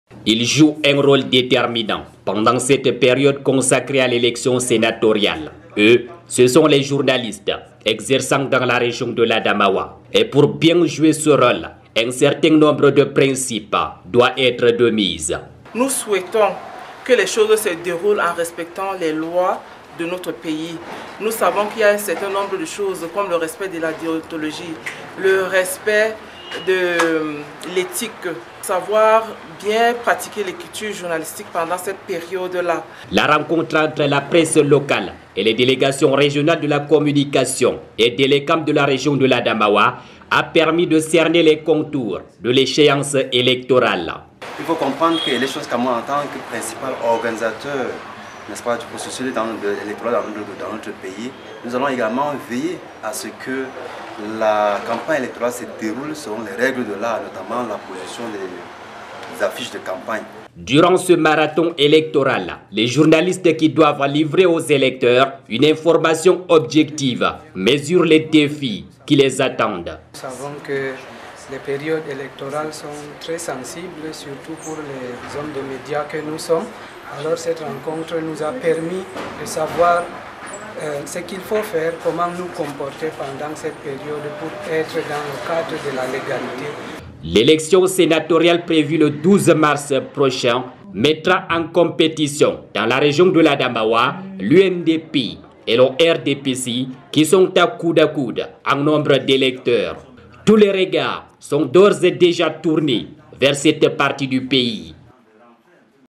LES JOURNALISTES AU FAIT DES ENJEUX ÉLECTION SÉNATORIALE DANS L'ADAMAOUA AU CAMEROUN
La rivalité entre les 2 partis UNDP et RDPC ne tient qu'à un fil dans région de l'Adamaoua. Les protagonistes sont au coude-à-coude pour les grands électeurs appelés à choisir les représentants au sénat. Les enjeux du scrutin dans l'Adamaou fait l'objet de la rencontre entre les journalistes et Elecam sous la supervision Christine Baria délégué régional communication...